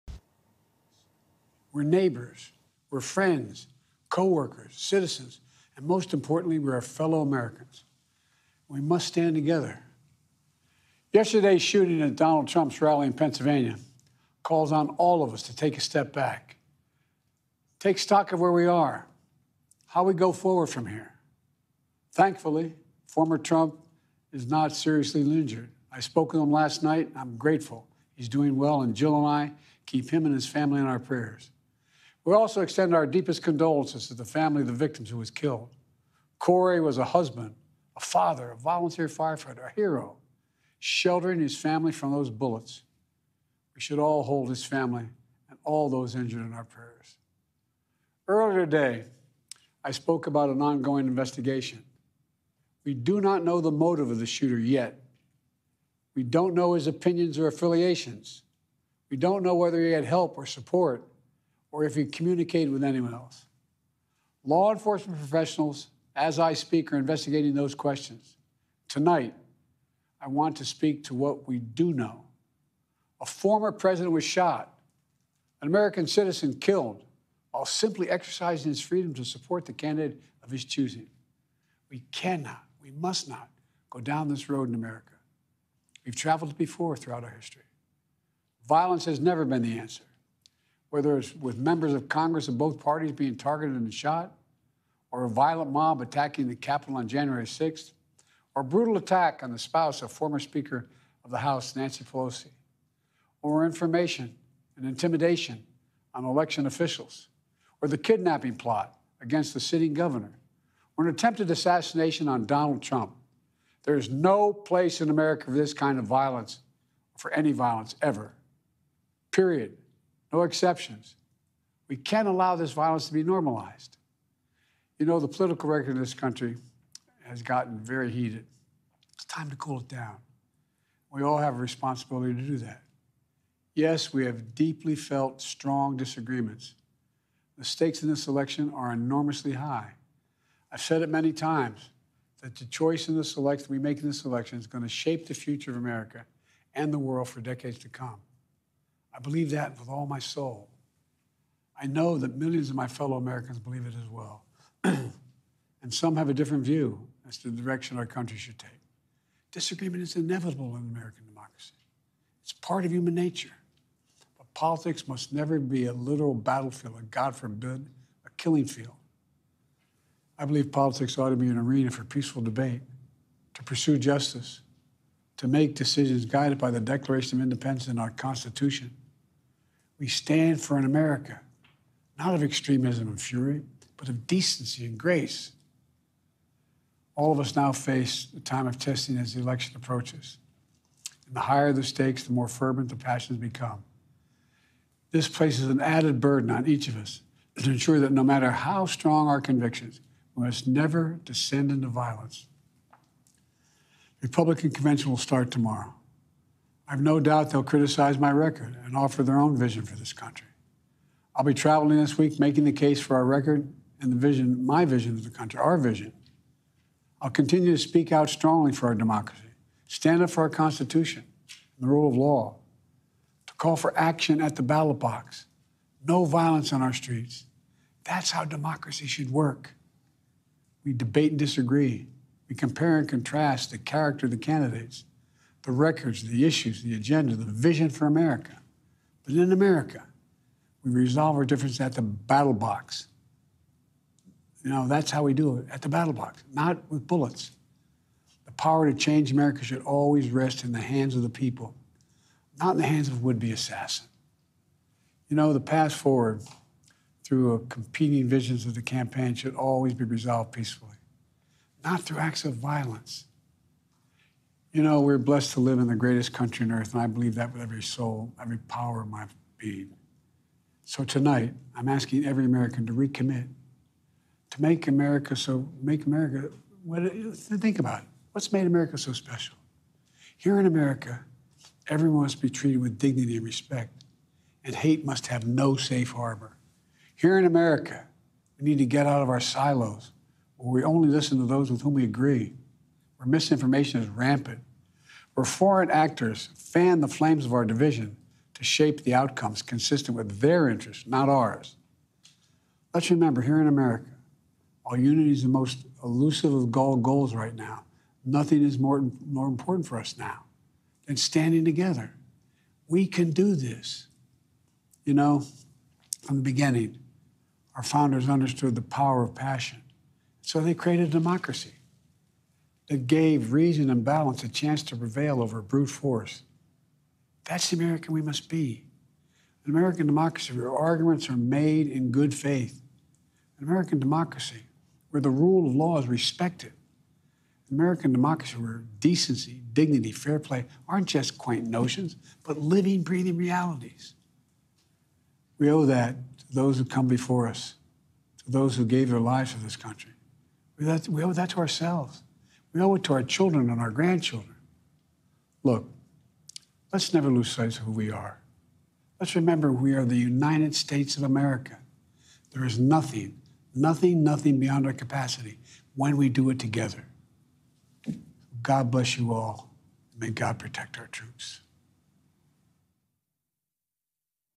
Biden pushes for unity after Trump assassination attempt
U.S. President Joe Biden is calling for unity after Trump was the target of an assassination attempt.